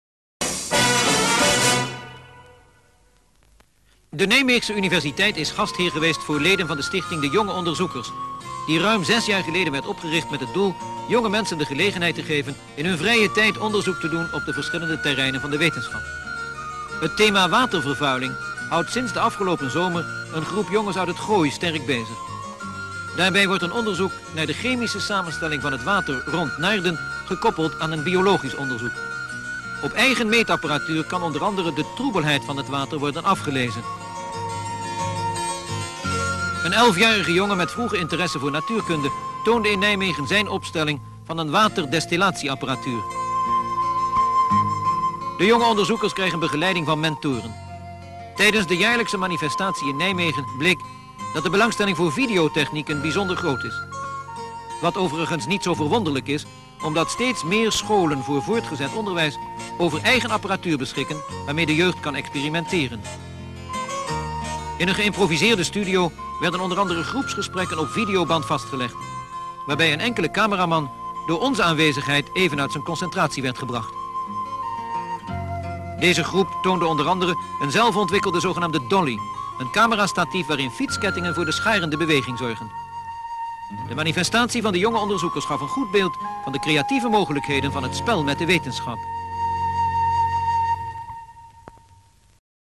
Landelijke manifestatie van de stichting 'Jonge Onderzoekers'
Bioscoopjournaals waarin Nederlandse onderwerpen van een bepaalde week worden gepresenteerd.
De Stichting Jonge Onderzoekers houdt op de Nijmeegse Universiteit hun jaarlijkse manifestatie. De stichting is zes jaar eerder opgericht met als doel jonge mensen de gelegenheid te geven onderzoek te doen op verschillende terreinen van de wetenschap. SHOTS: ext. Universiteit Nijmegen; overzicht van de stands, met o.a. een onderzoek naar watervervuiling; jongeren die experimenteren met filmcamera's en een filmcamera op een dolly waarmee de camera op en neer bewogen kan worden.